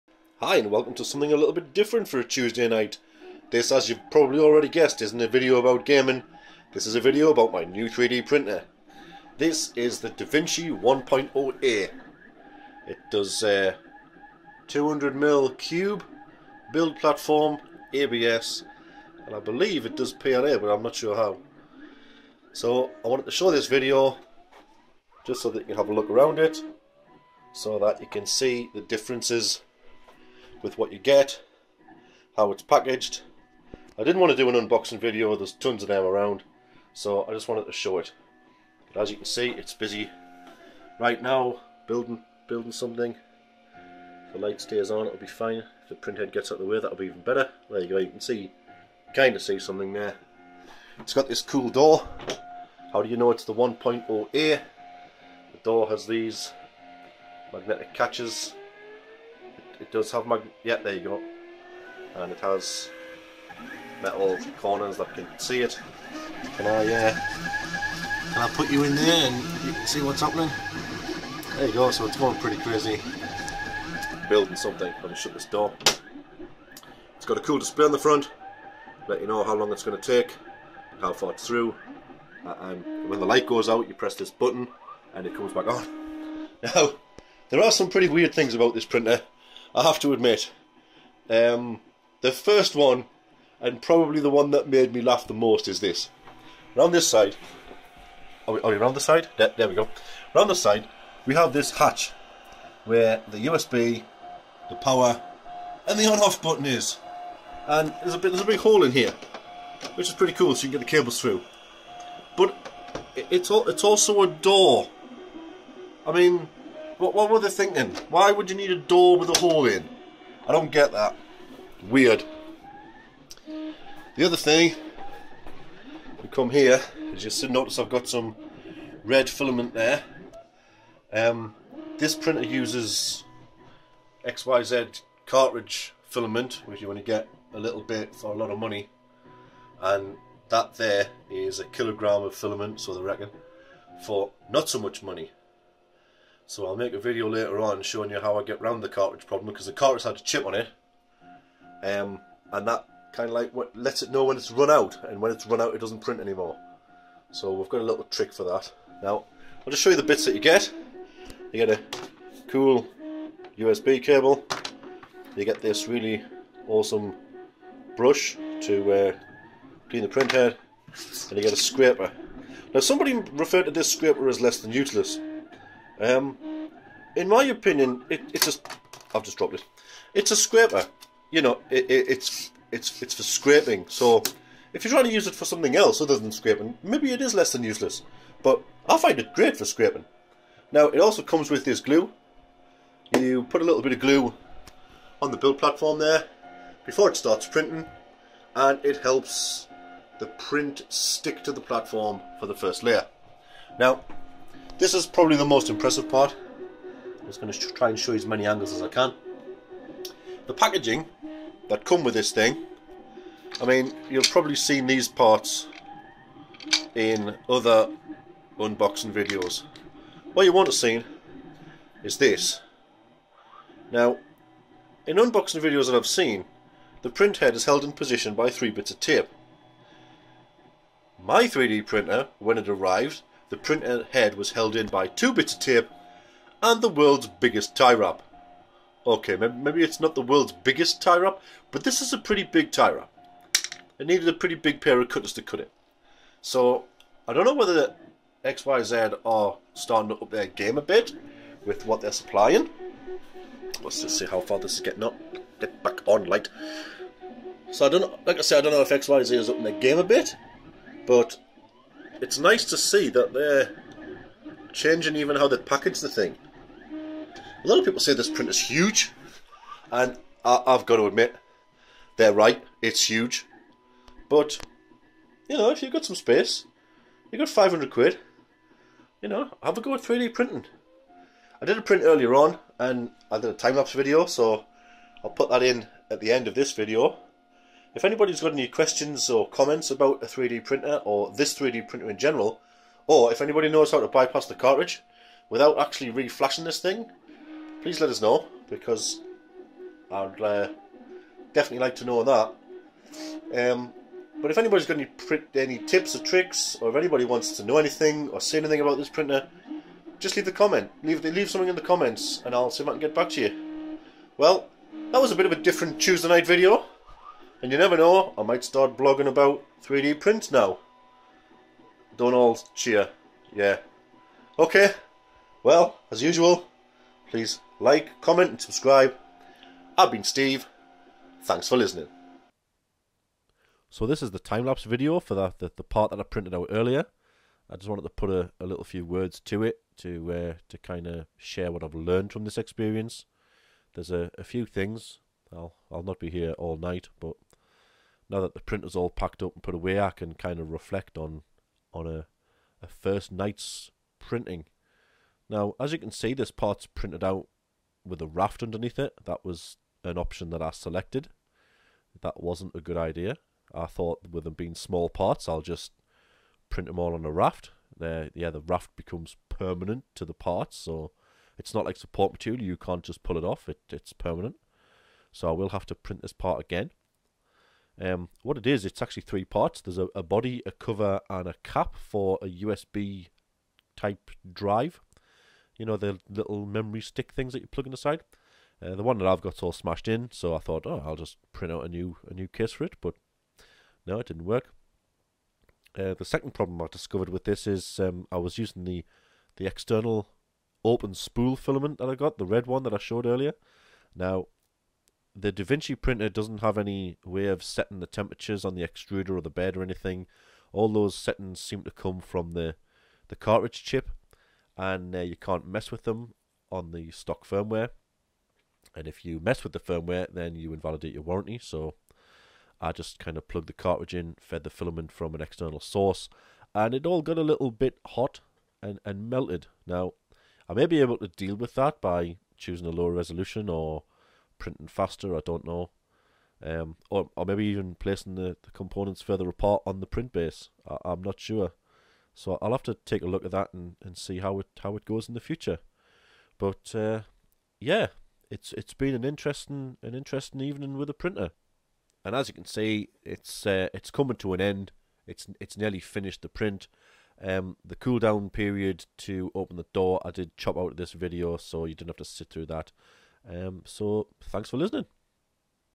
XYZ DaVinci 1.0A 3D Printer
The world of 3D printers is an exciting place.  I finally picked up a budget printer so I could get started.  Looks like there's lots to learn, but I am sure I'll get there.  I plan to video everything I learn and share it with you all.

Sorry about the poor quality of this video, I used my phone of all things to film it.  Hmmm maybe I'll print a tripod mount for my phone some time ha ha ha.  Enjoy :)